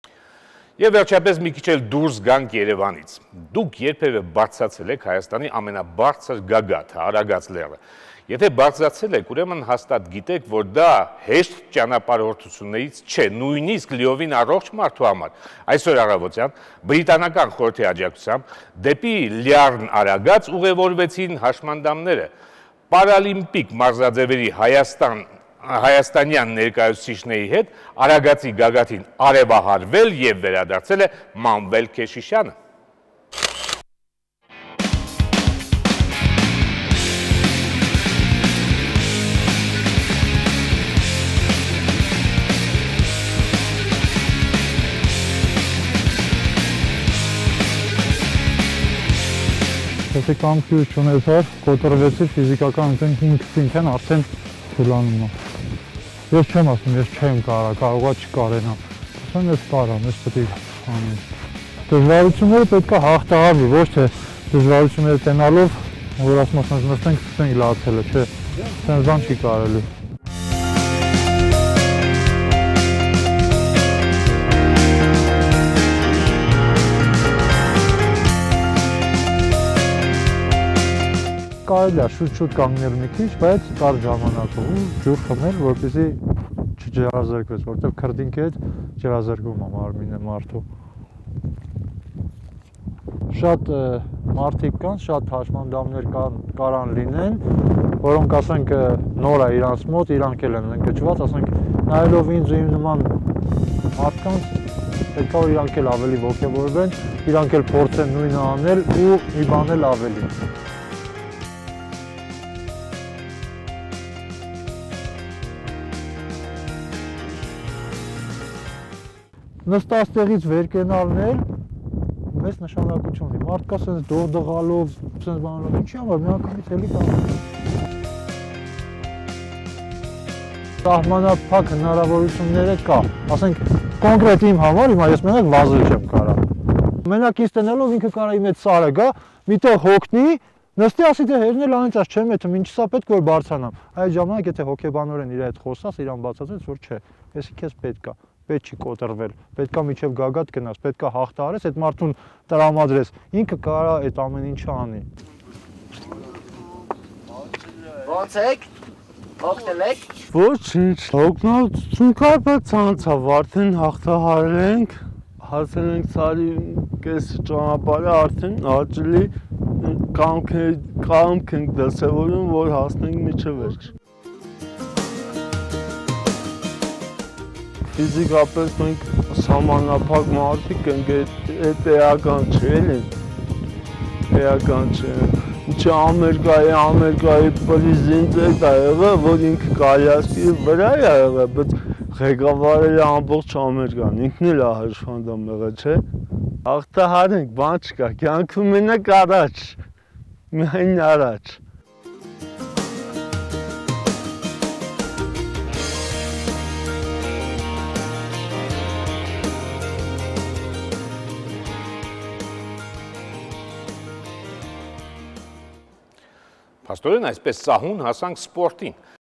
Climbing Aragats ( A story covered by Shant TV)
In countdown to the opening of London 2012 Paralympics, the representatives of the British Council Armenia, the British Embassy Yerevan, Armenian Paralympic Committee and Samsung Representation climbed the Aragats to celebrate the Games. A story covered by  Shant TV.